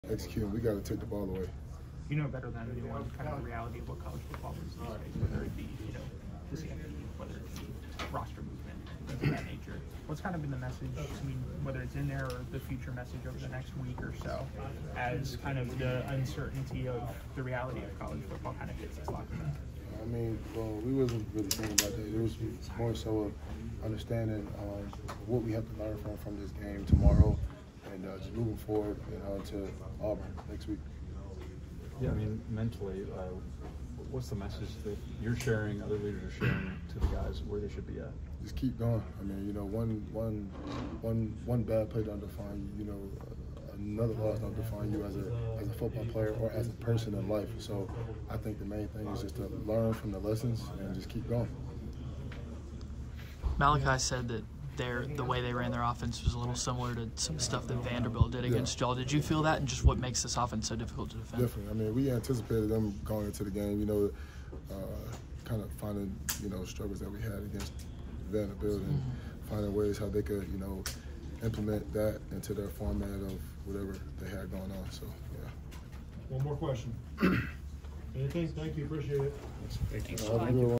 Alabama Football LB Jihaad Campbell Interview | Oklahoma Postgame
Alabama Football linebacker Jihaad Campbell discusses the Crimson Tide's 24-3 loss on the road at Oklahoma on Saturday night in Norman.